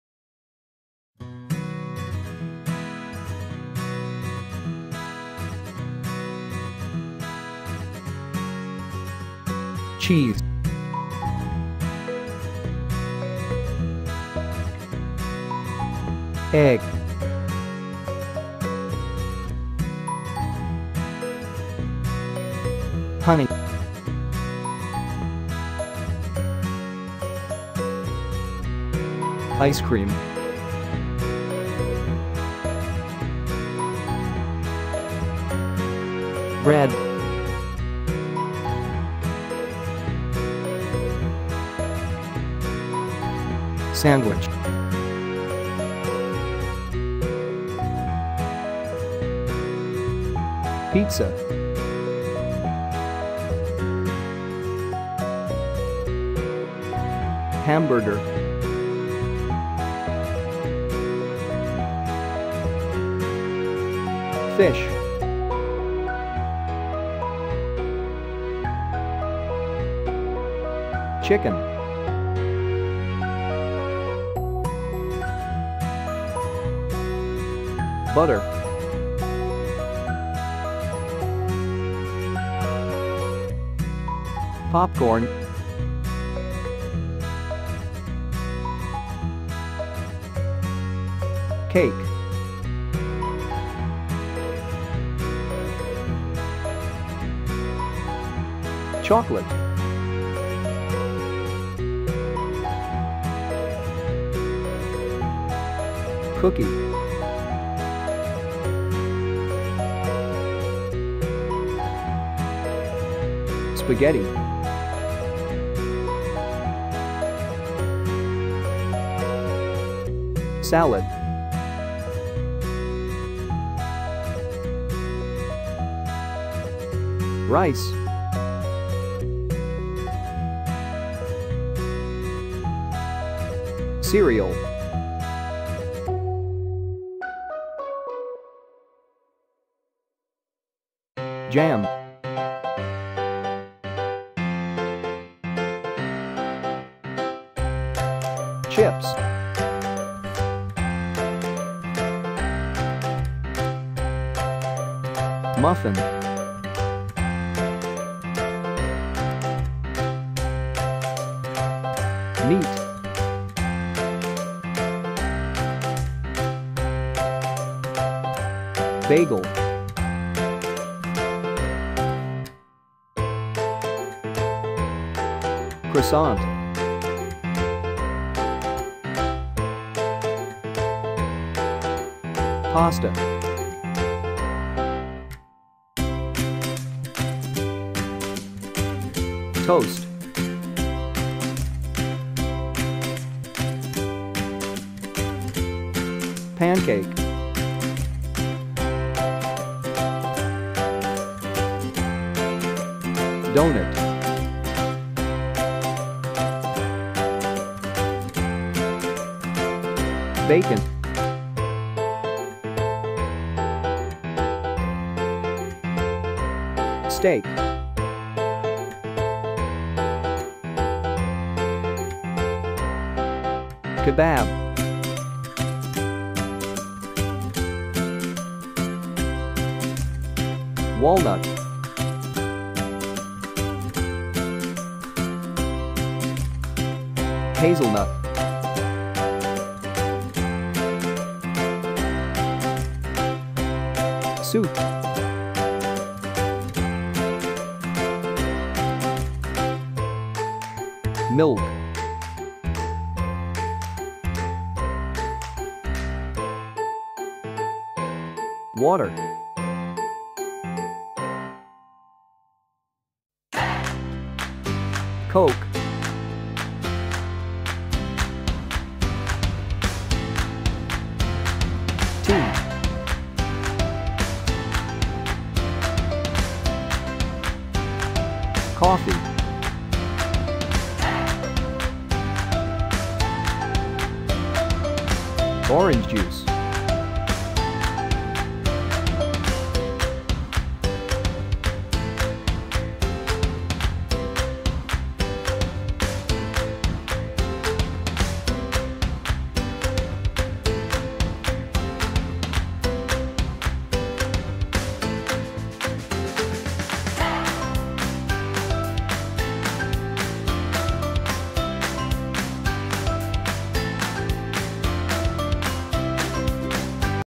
LET'S LEARN - FOOD AND DRINKS (İNGİLİZCE YİYECEK VE İÇECEKLER)
LEARN THE NAMES AND PRONUNCIATION OF FOOD AND DRINKS.

THE MOST COMMON/POPULAR FOOD AND DRINKS IN ENGLISH

(İNGİLİZCEDE YİYECEK & İÇECEK İSİMLERİ VE OKUNUŞLARI )